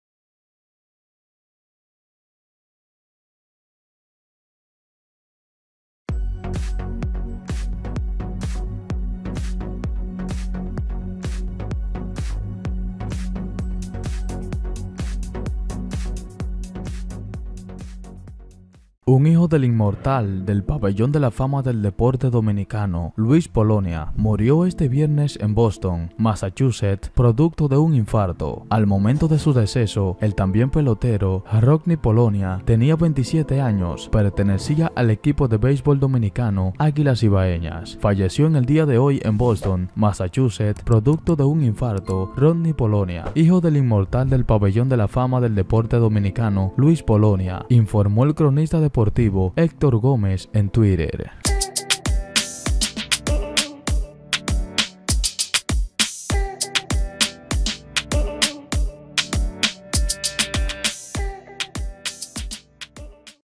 Fallece de un infarto Rodney Polonia, hijo del exjugador Luis Polonia
Rodney Polonia, hijo del legendario bateador de la Liga Dominicana de Béisbol Luis Polonia, falleció este viernes a los 27 años de edad luego de ser víctima de un infarto en la ciudad de Boston Massachusett, según informó Héctor Gómez en su cuenta de Twitter.